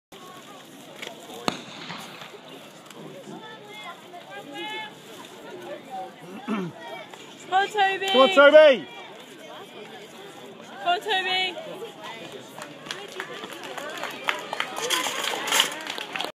Tobie 75m hurdles
Trafford Open 21.4.14
Heat 1 U13 boys
Tobie ran a pb of 13.8